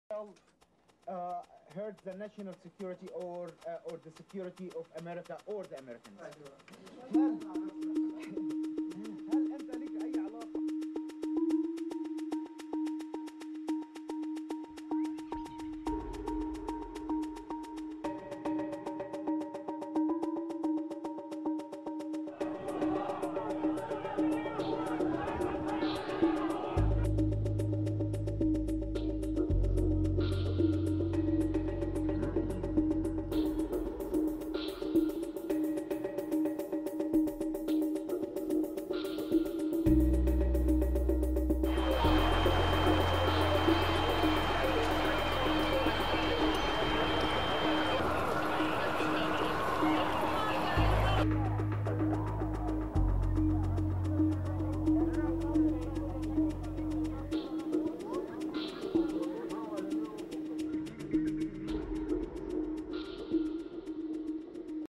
Omar Abdel-Rahman, 'Blind Sheik' in 1993 Trade Center Attack, Dies
Omar Abdel-Rahman, architect behind the 1993 truck bombing of New York’s World Trade Center, died Saturday at a federal prison of natural causes. He was 78. Photo. Getty.